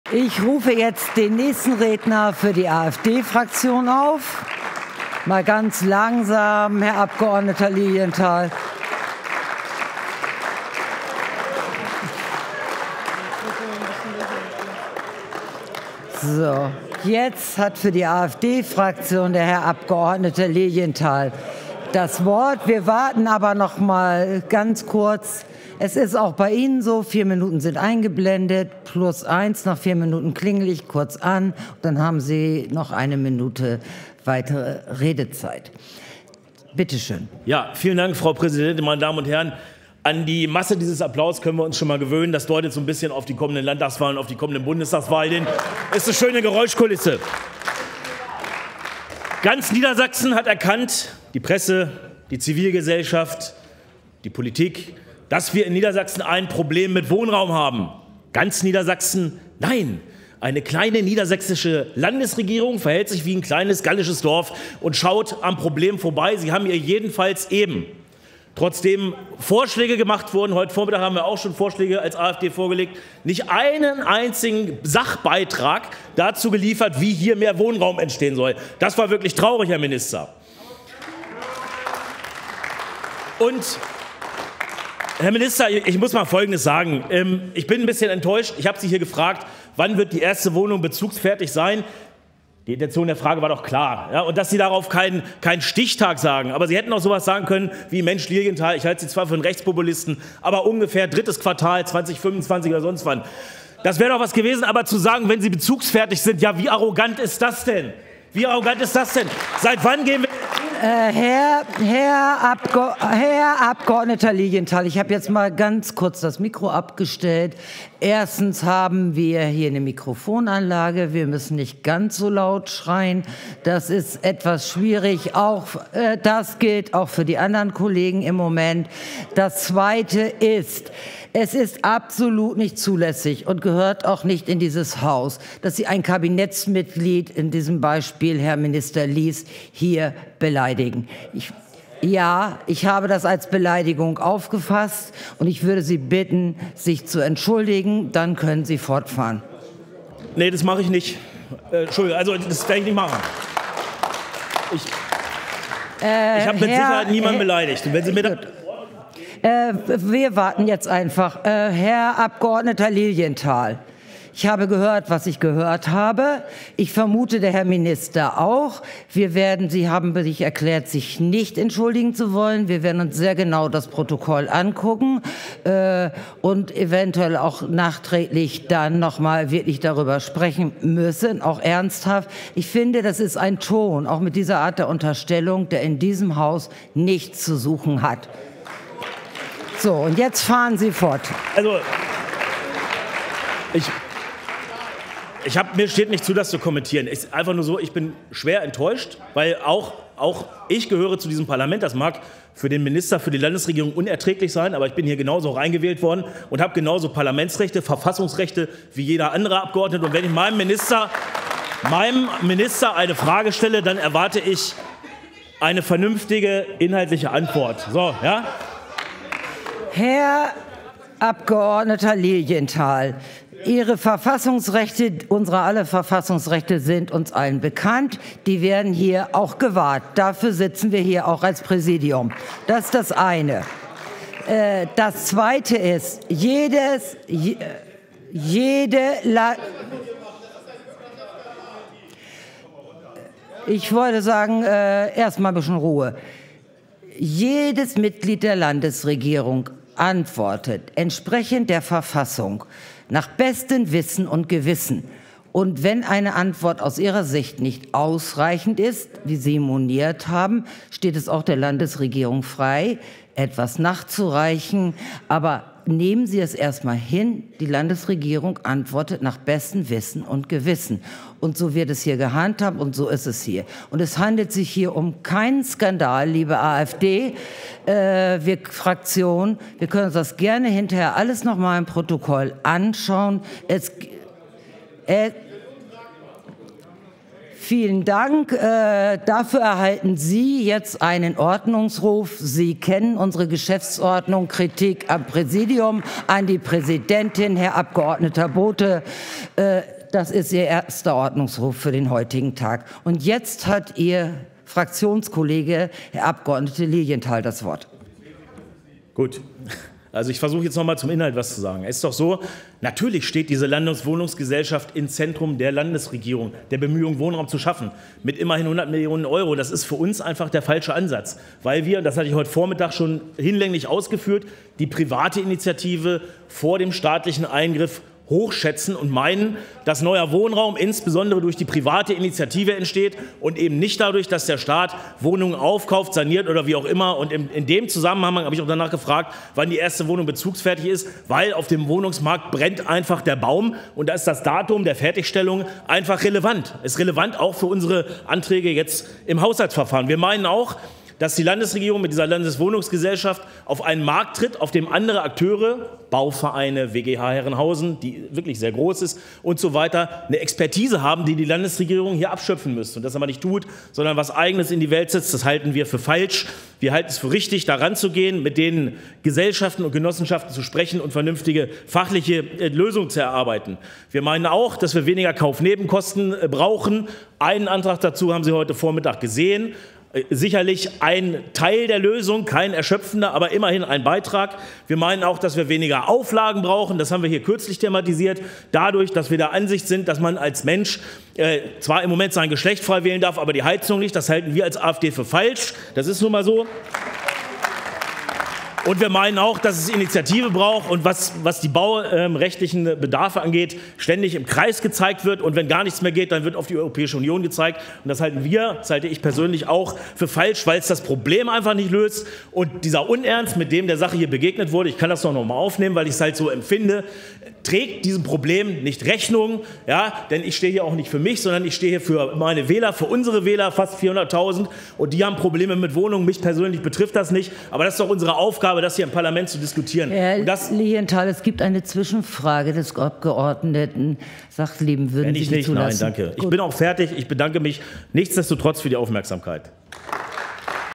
Unfassbare Entgleisung grüner Präsidentin bei AfD-Rede! – Peer Lilienthal (AfD)
Unterbrochen, Mikrofon abgedreht, beschuldigt, wieder unterbrochen, belehrt: Die Präsidentin Meta Janssen-Kucz (Grüne) greift AfD-Redner Peer Lilienthal an – und am Ende soll ER sich entschuldigen. Man muss es sehen, sonst glaubt man es nicht.

Ja, das sind inzwischen die Zustände in einem deutschen Parlament. Wir haben viele Zuschriften bekommen und selbst aus anderen Fraktionen im Landtag hieß es: Was da abgezogen wurde, geht gar nicht. Was denken Sie?